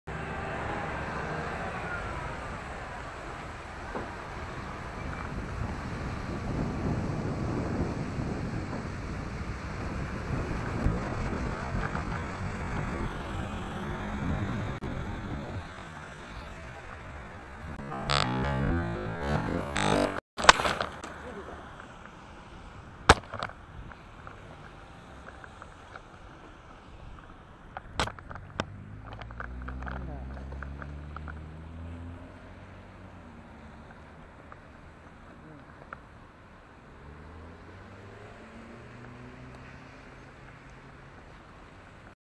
あるある ロードバイク事故 転倒
あるある転倒事故

原因　タイヤを雨グリップに弱いのに替えていたのを忘れていた

普通に減速が不十分だったが、今までのタイヤなら何とかカウンター当てて曲がれる感覚はあったが、思い込みだった

そもそも直角カーブで曲がる速度ではない。

1年で一万キロ乗りますが、今年初コケでした。

反省します！